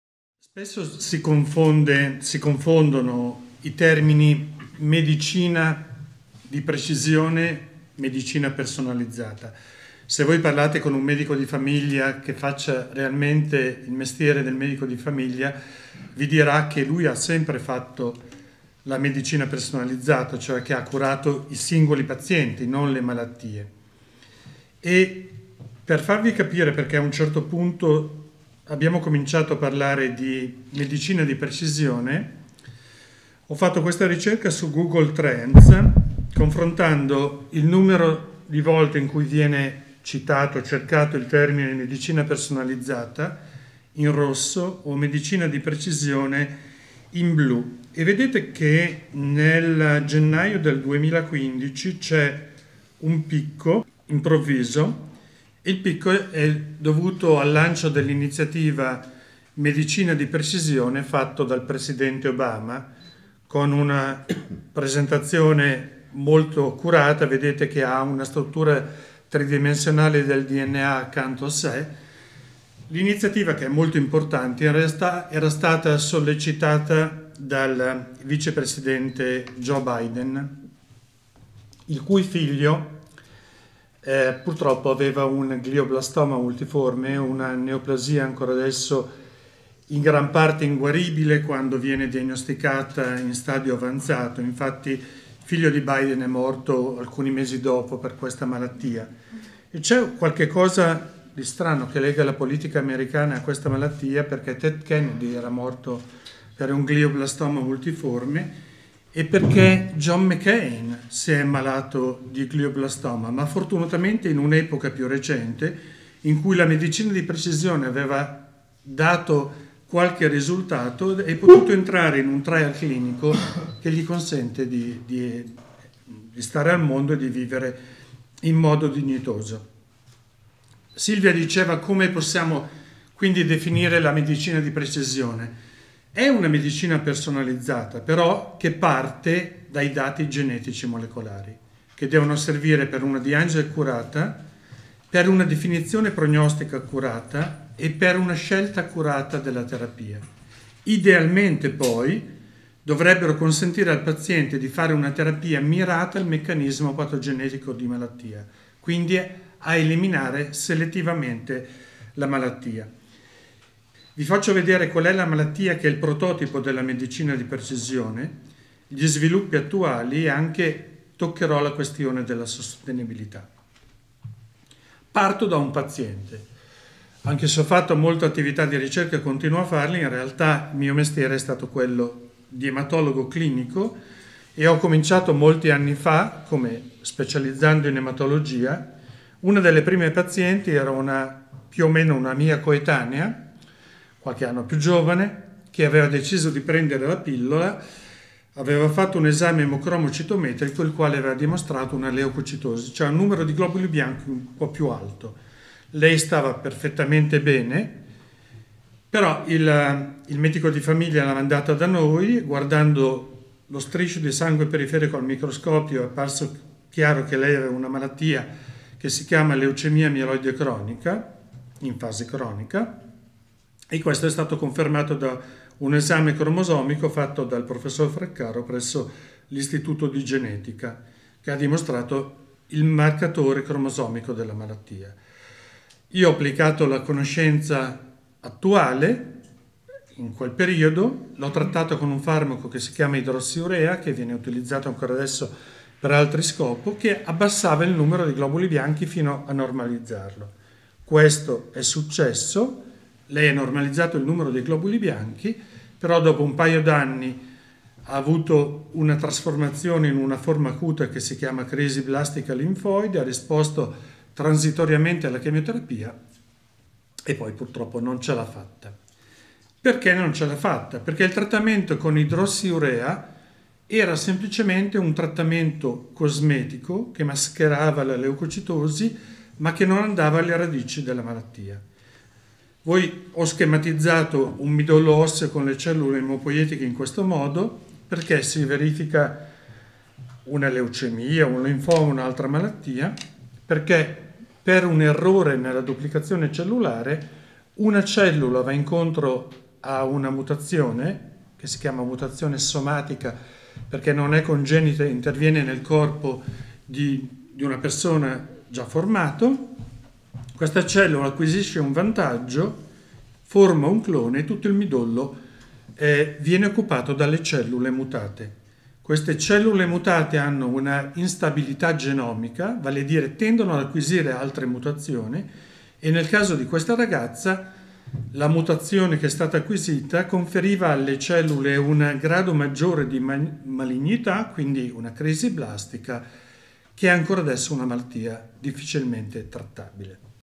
La struttura della medicina di precisione #2/20
Contents:
Mario Cazzola, Università di Pavia e Fondazione IRCCS Policlinico San Matteo
Part: 2/20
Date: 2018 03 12
Place: Fondazione Giannino Bassetti, Milano.
Language: Italiano.